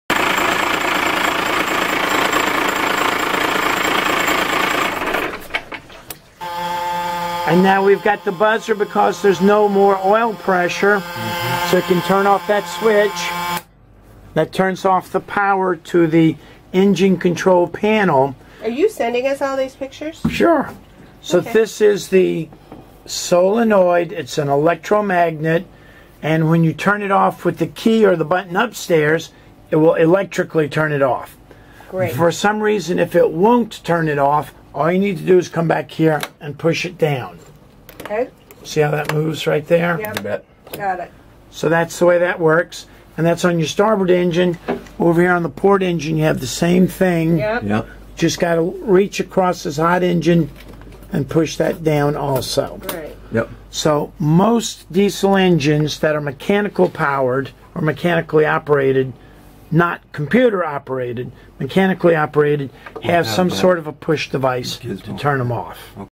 Manual STOP lever, diesel engine
STOP, the engine will NOT STOP. What do I do? Find the manual stop lever and push it with your finger. Or if an electronic diesel push the RED STOP button. Learn your systems here in Vero Beach, FL.